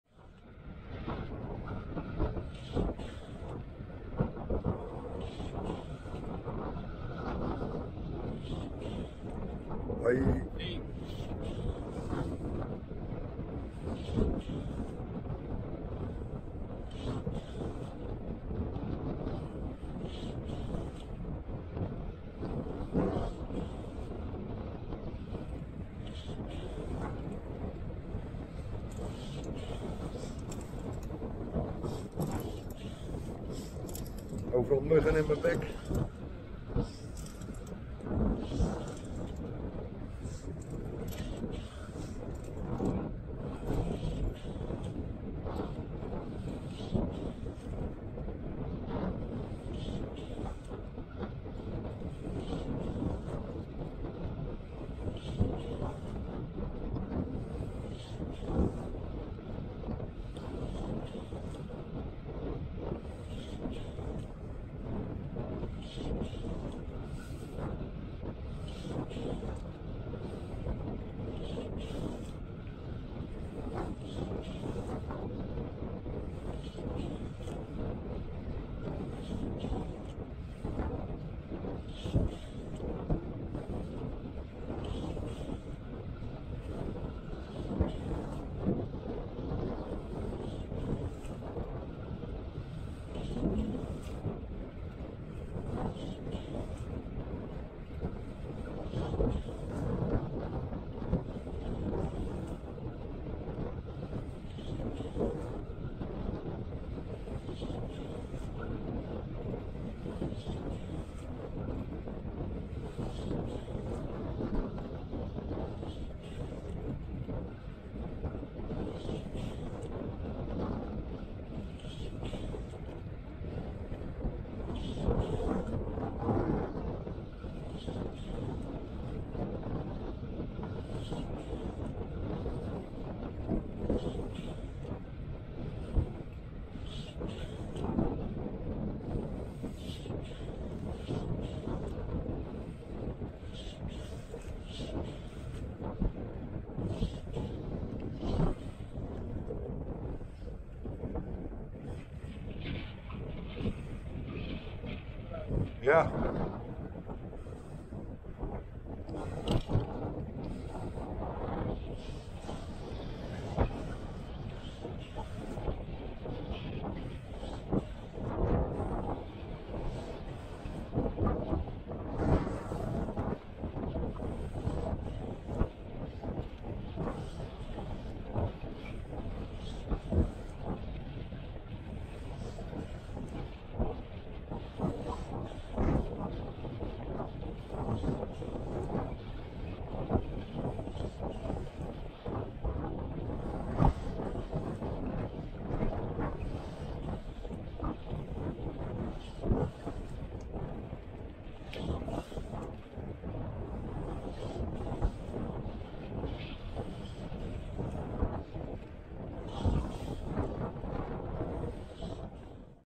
Wellicht het smalste fietspad van Noord Beveland.
Langs het Veerse meer op de rowingbike, smal fietspad tussen het haventje van Geersdijk en de polder bij Kamperland.

Gelijk een mooie test met de nieuwe actioncam